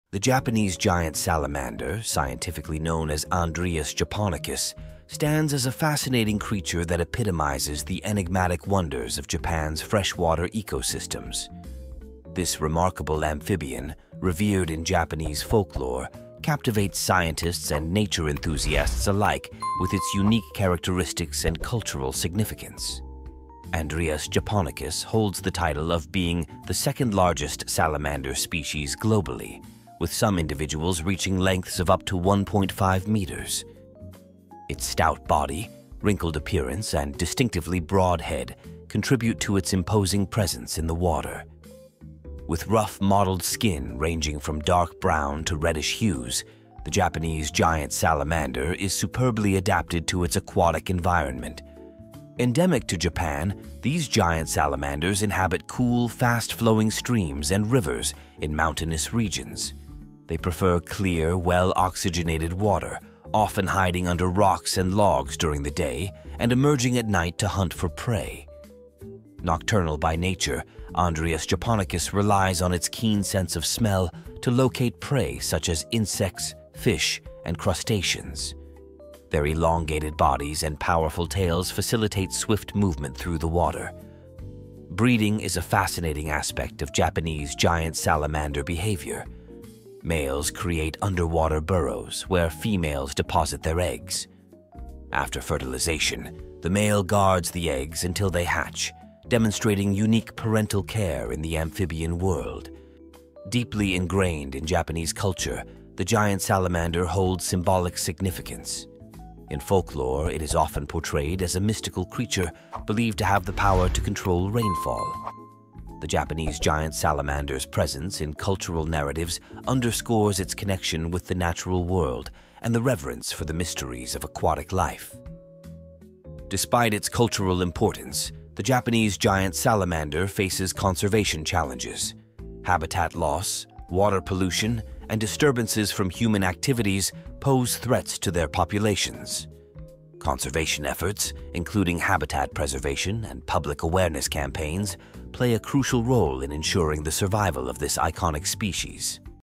Andrias Japonicus: Unveiling the Mysteries of the Japanese Giant Salamander
🦎 Dive into the enchanting world of Andrias japonicus, the Japanese Giant Salamander! Discover the secrets of this aquatic giant, from its impressive size and unique features to its cultural significance in Japanese folklore. Explore the pristine streams and hidden habitats where these mystical creatures thrive, showcasing the delicate balance of nature. Join us on a journey through the waters of Japan to unveil the mysteries of the Japanese Giant Salamander. 🌊🇯🇵